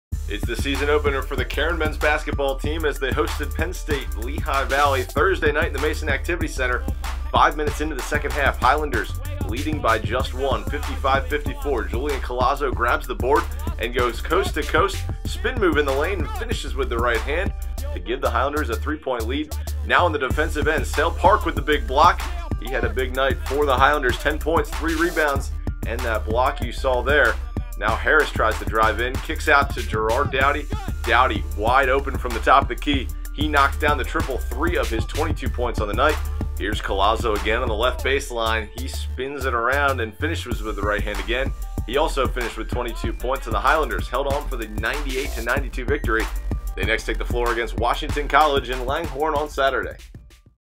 Cairn Men's Basketball Highlight of the Night - Nov. 17, 2016
The Highlanders won their Season Opener for the second-straight season with a 98-92 victory over Penn State - Lehigh Valley.